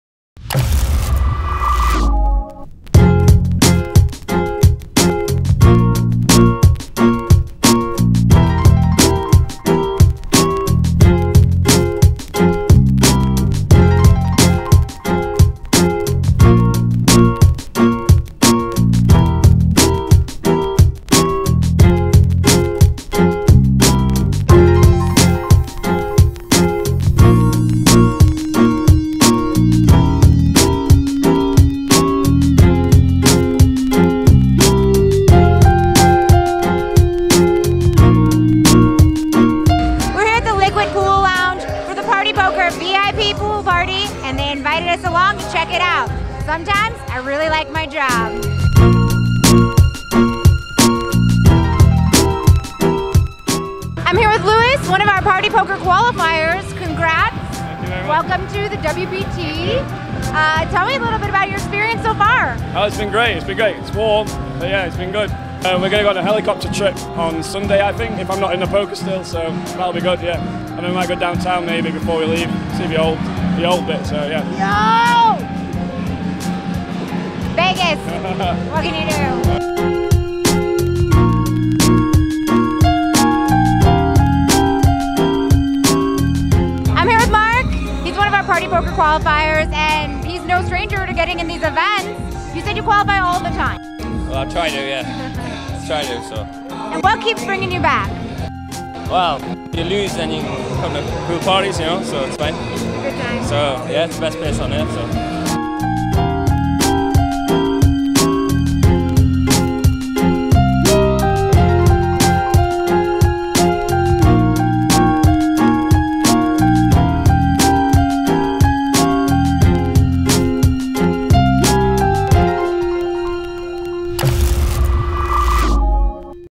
Season XIV partypoker presents WPT500 at ARIA Resort and Casino: partypoker Pool Party
partypoker qualifiers partied it up at Liquid Pool Lounge at the ARIA Resort and Casino before they take their shot in the WPT500.